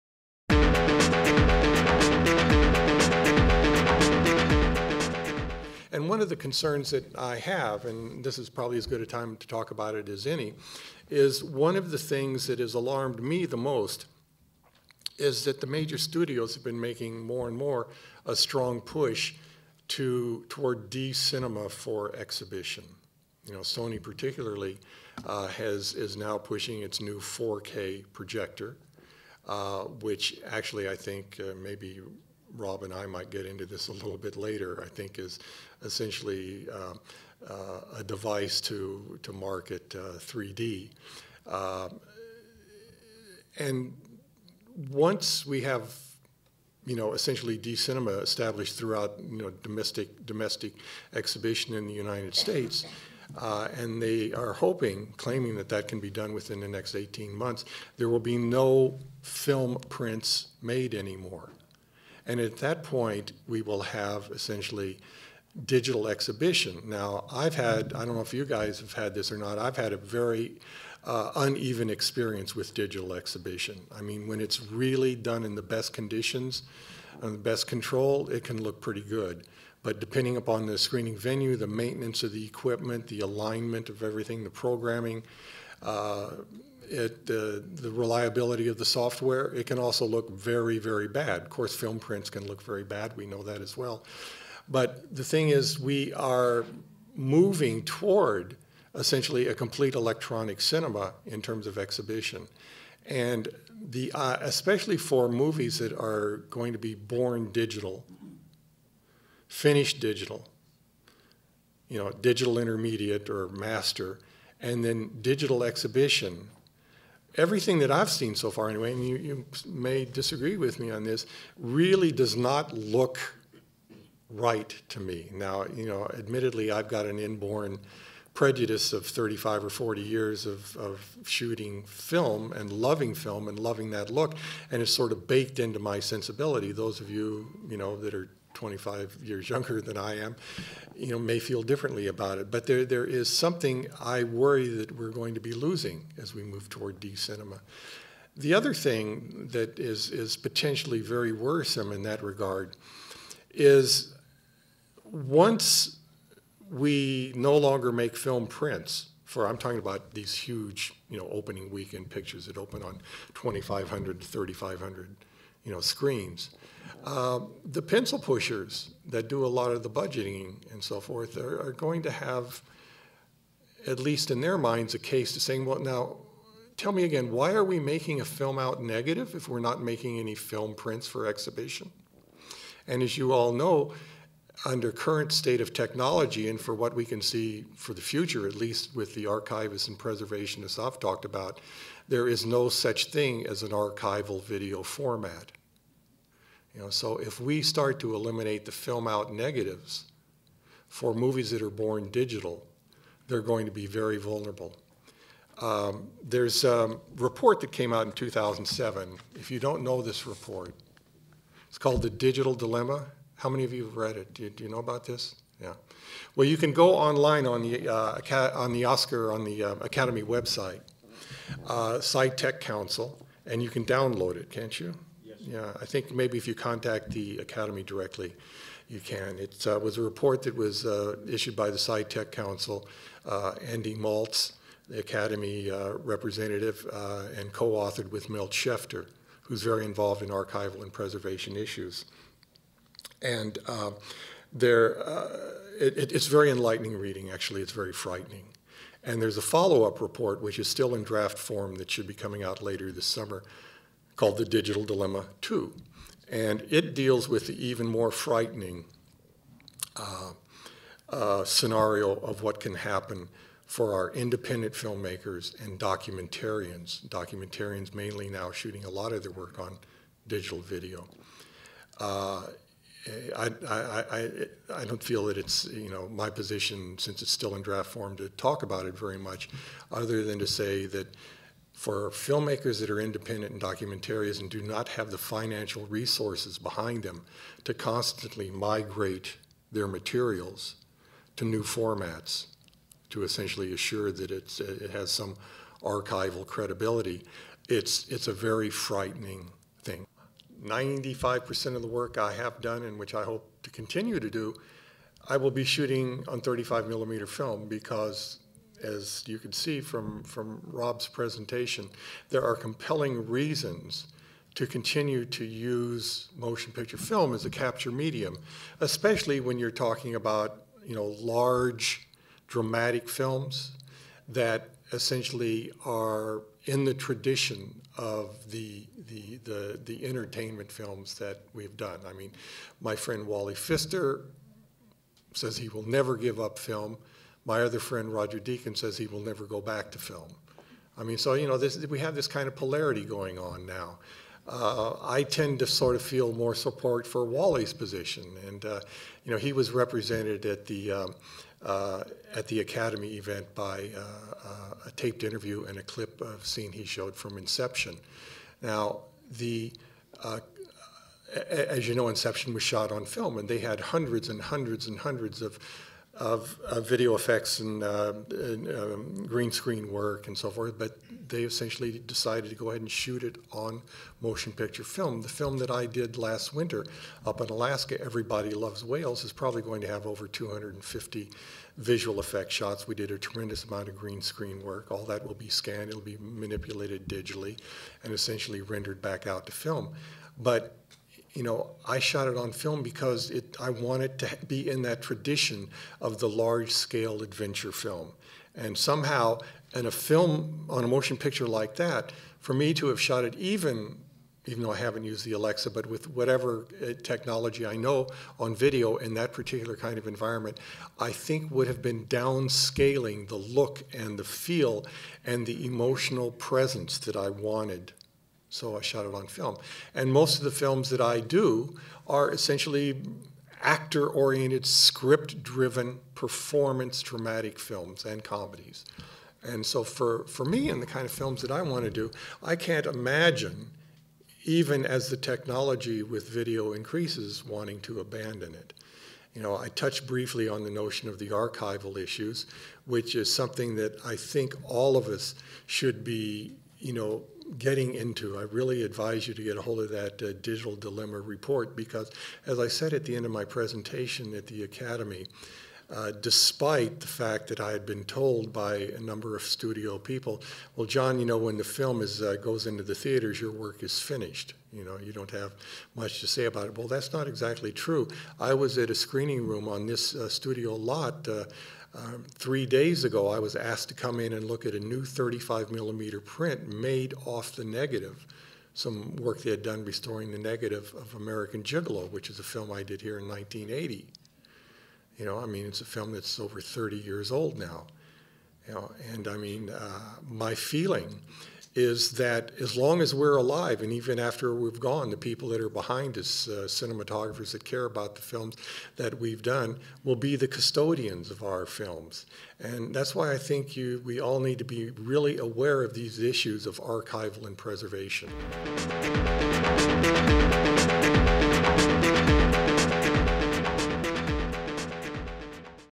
Kodak Panel with John Bailey, ASC at CineGear Expo 2011
Kodak Panel with John Bailey, ASC at CineGear Expo 2011. John shares his thoughts on digital exhibition and workflow, the Digital Dilemma Report, and film archiving .